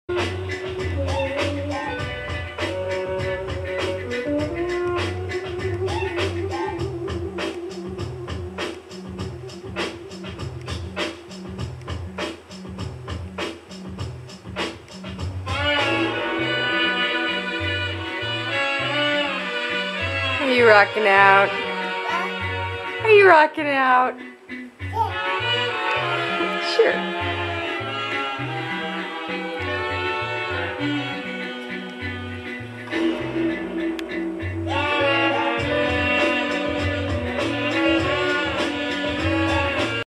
Balkan Beat Baby
Roh LOVES all kinds of music. Here he is "rocking" out to Balkan Beat Box @ 14mos old.